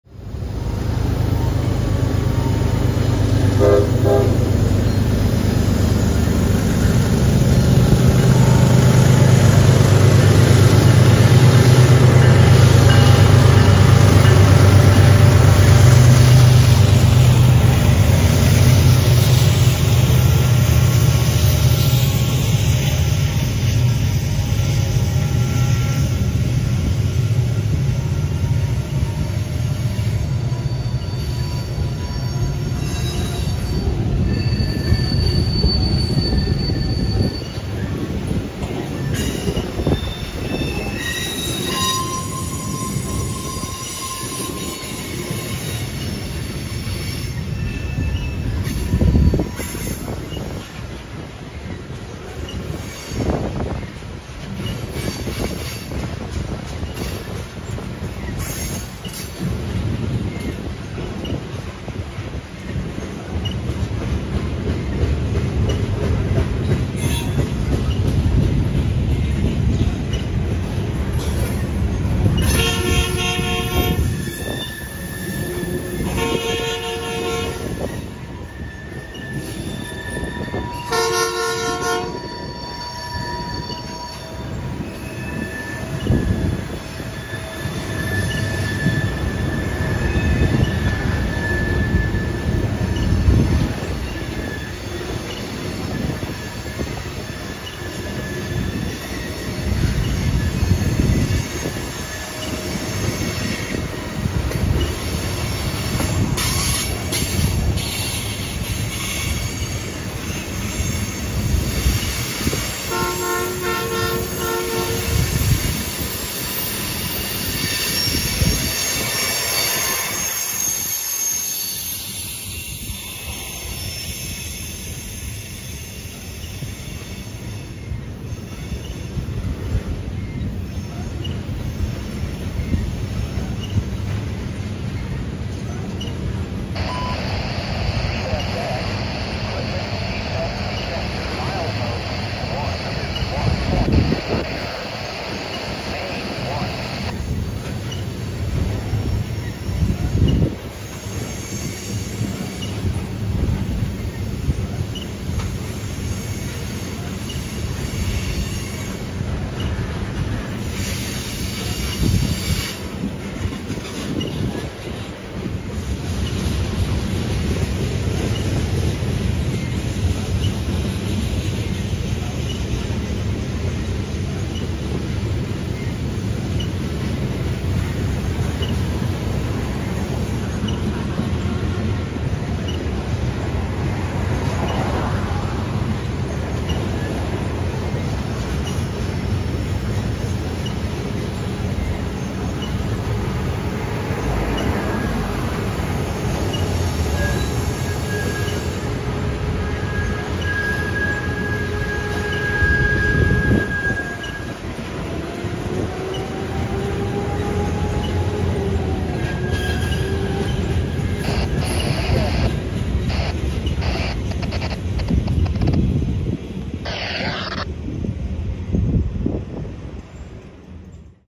Tanks for the Memories
A disguised CSX B729 with Baltimore-bound ethanol attacks the hill on the B&O New Castle Sub in Akron, OH. The LyondellBasell critter added some flavor. Thankfully this trio “pushed” a cloud into this mid-day scene